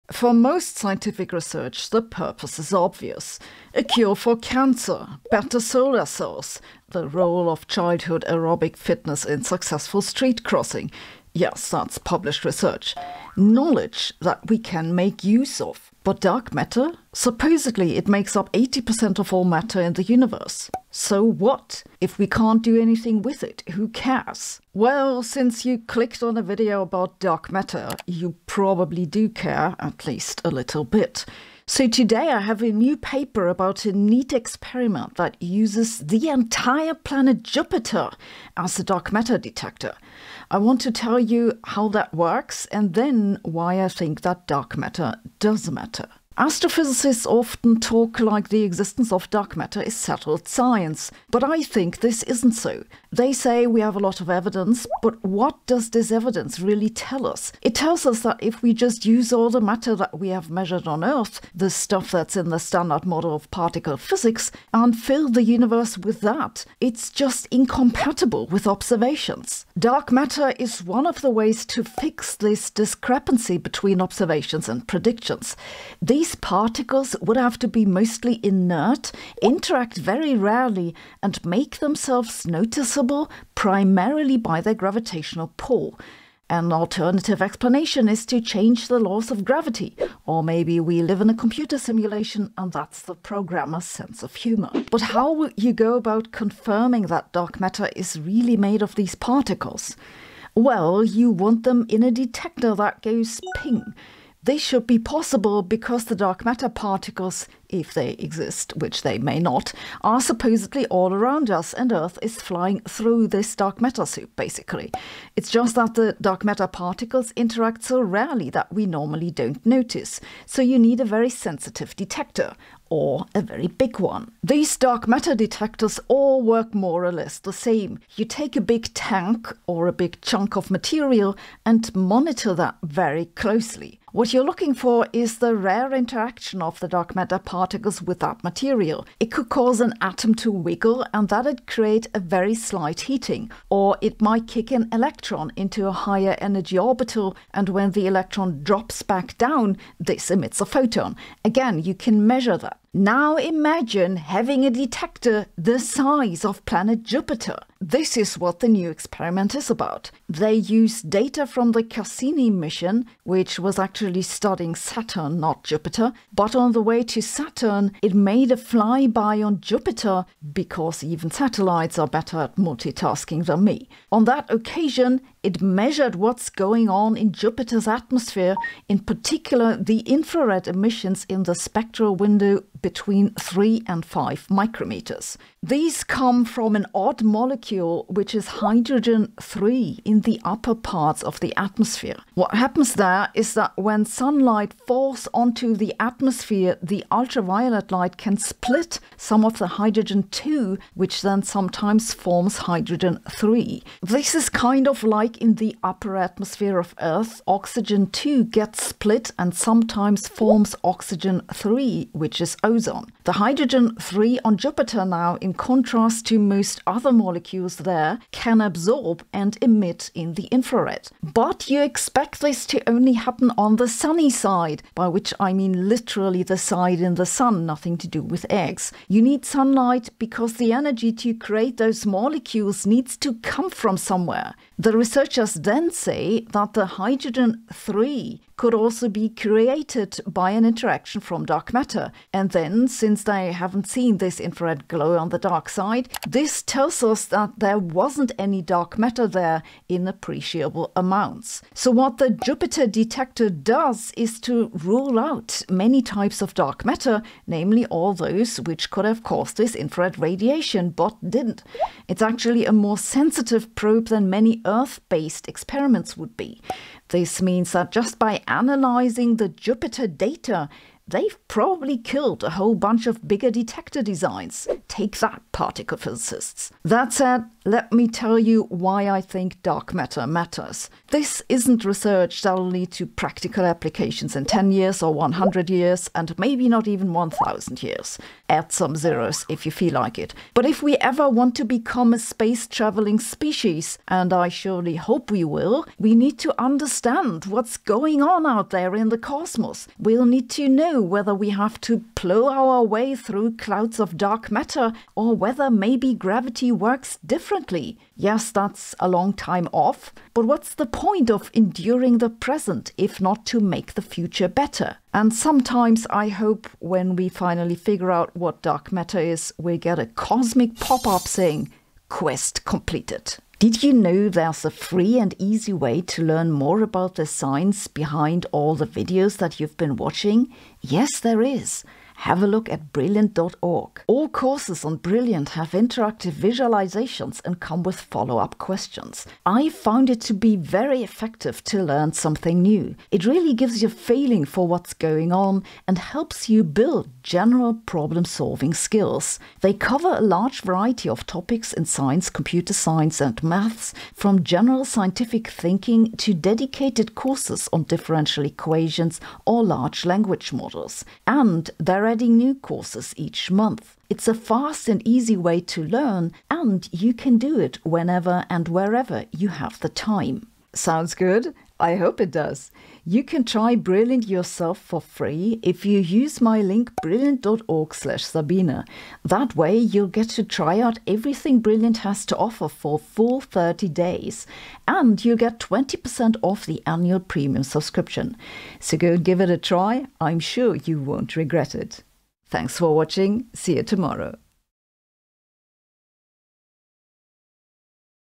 ANOTHER dark matter experiment finds nothing -- Why keep doing it?
80% of all matter in the universe is made up of dark matter, astrophysicists say. Yet they can't seem to find any evidence for the particles that dark matter is supposedly made up of. A new experiment used the entirely planet Jupiter as a dark matter detector. It didn't find any evidence of the stuff, but this will make some particle detectors on earth superfluous. Let’s have a look.